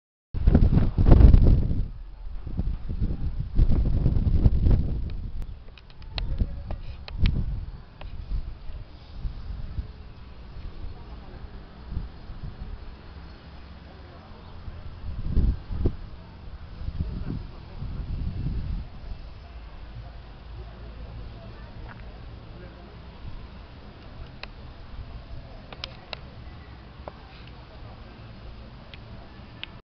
SALINA DA CRUZ- TERRENO PLANO PARA CONSTRUÇÃO DE NOVAS CASAS EM JULHO DE 2012
Em julho de 2012, foi iniciada a construção de 30 casas do PROGRAMA MINHA CASA, MINHA VIDA -  às quais, foram concluídas em fevereiro de 2014